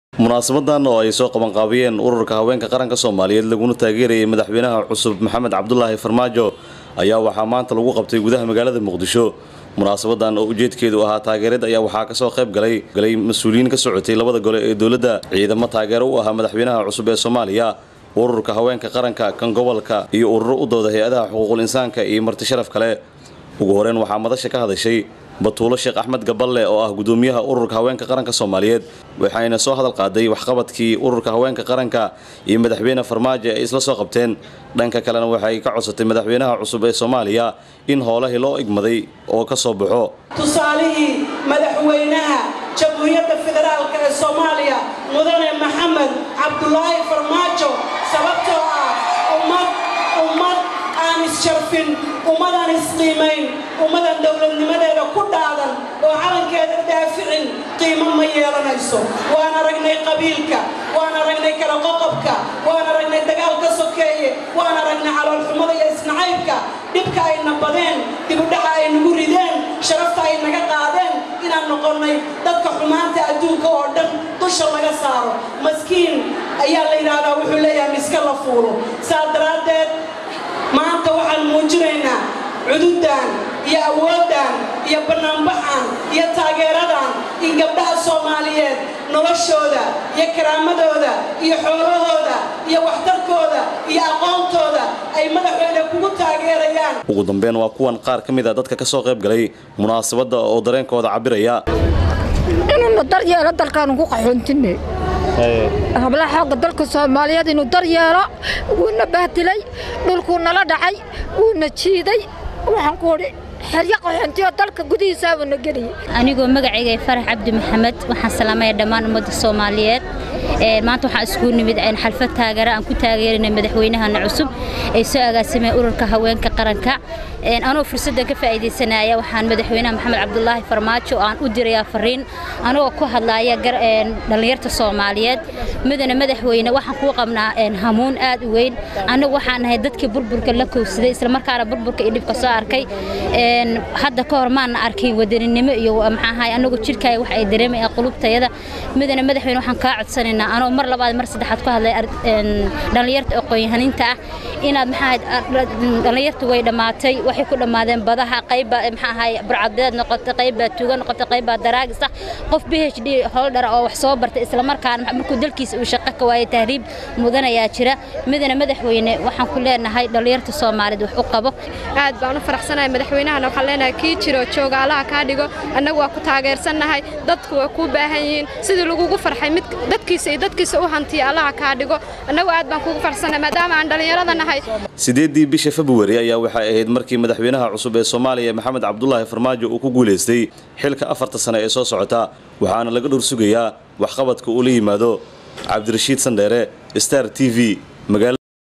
DAAWO FARMAAJO OO KULAN BALAADHAN LOOQABTAY
DAAWO KULAN BALAADHAN OO LOOQABTAY MADAXWEYNAHA FARMAAJO OO SI BALAADHAN LAGU SOO DHAWEEYEY WAX QABKIISA CUSUB